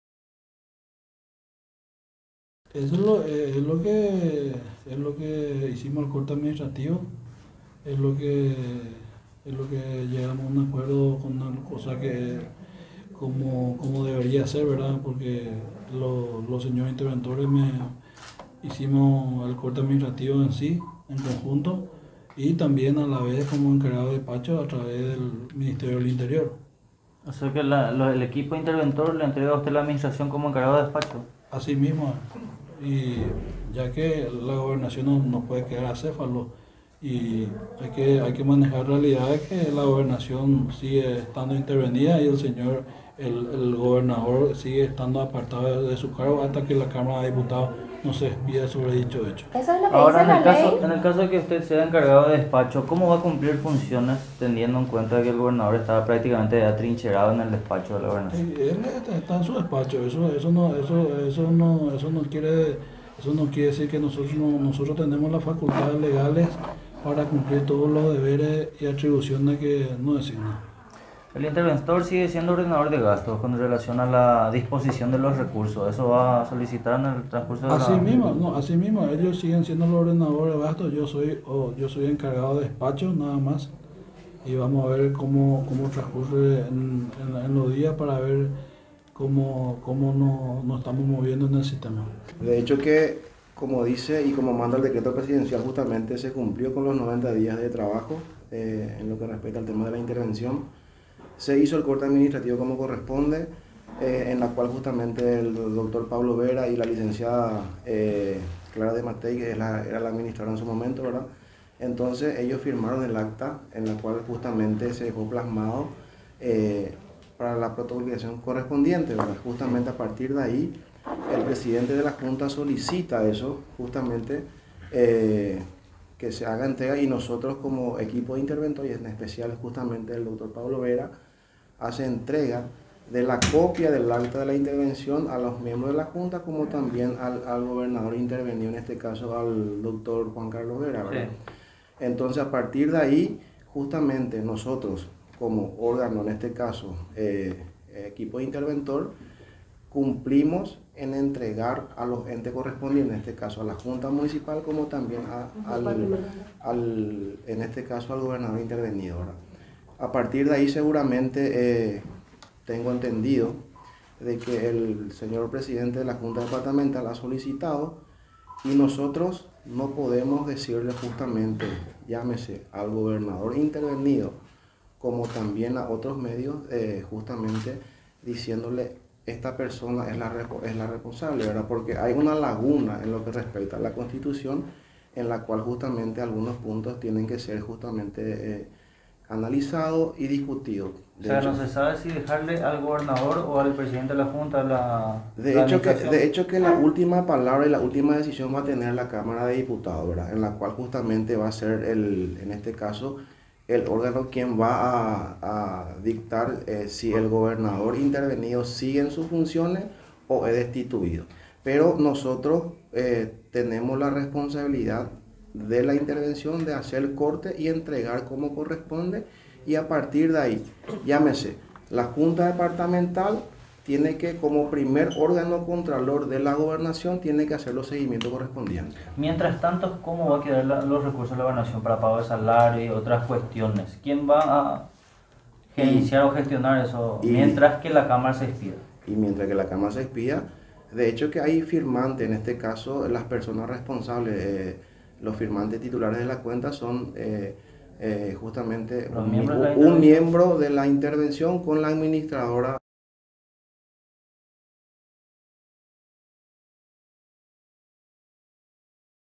Juan Rojas fue designado como encargado de despacho de la Gobernación
El Presidente de la Junta Departamental del Guairá, Juan Rojas Sanabria, informó que fue designado como encargado de despacho de la Gobernación, tras el corte administrativo que realizó el equipo Interventor. Aclaró que los interventores le entregaron la administración como encargado de despacho.